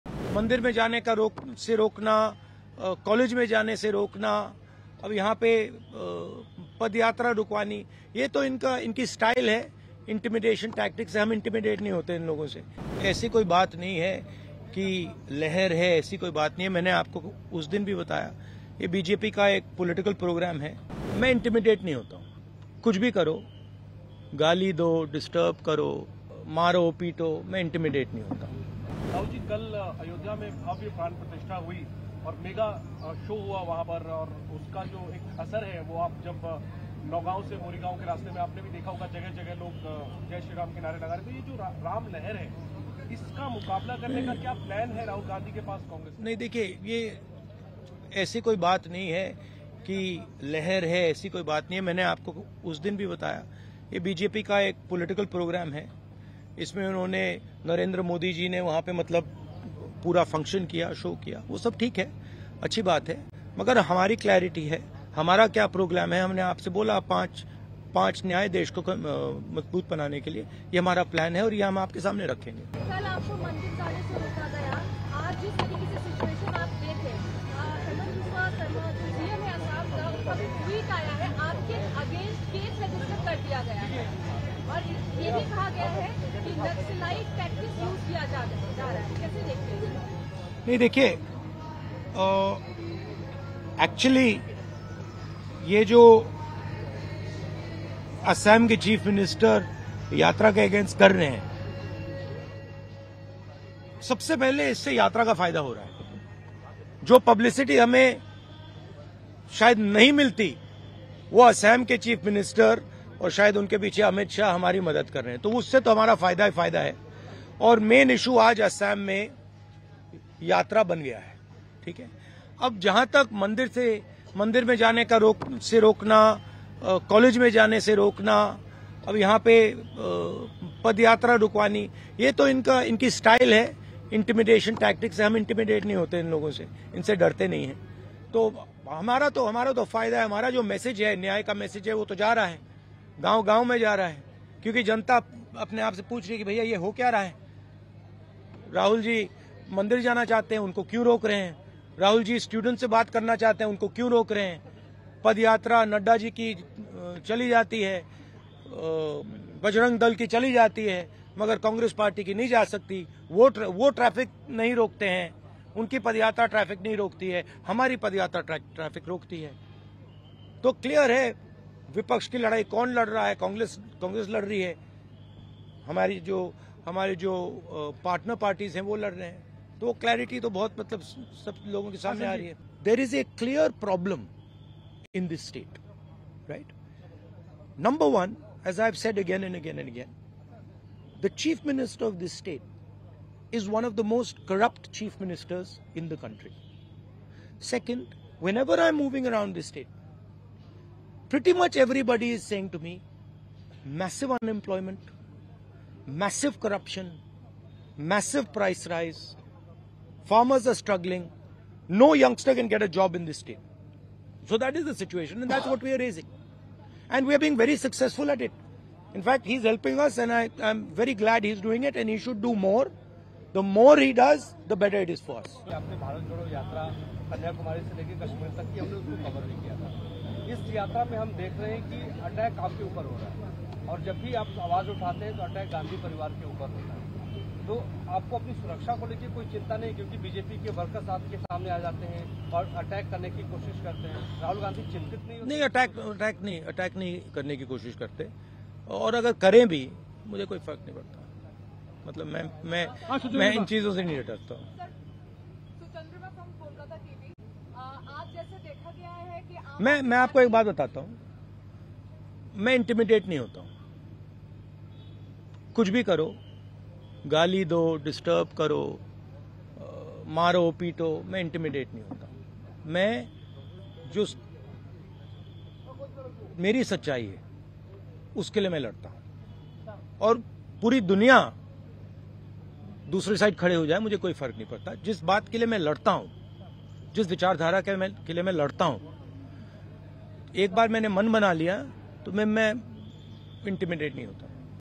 Rahul Gandhi On Ram Mandir | Ram Wave In India? Rahul Gandhi Says "BJP's Political Programme"
Congress leader Rahul Gandhi, whose ‘Bharat Jodo Nyay Yatra’ is currently in Assam, on Tuesday dismissed the presence of a “Ram wave” in the country. He told a press conference that the ‘Pran Pratishtha’ ceremony was the “BJP’s political programme”.